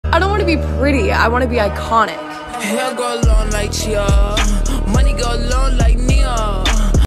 AR Girl in a Sherri World
We love this Sherri Hill cocktail dress on one of our AR Girls! Video taken by Brittany Link Photography!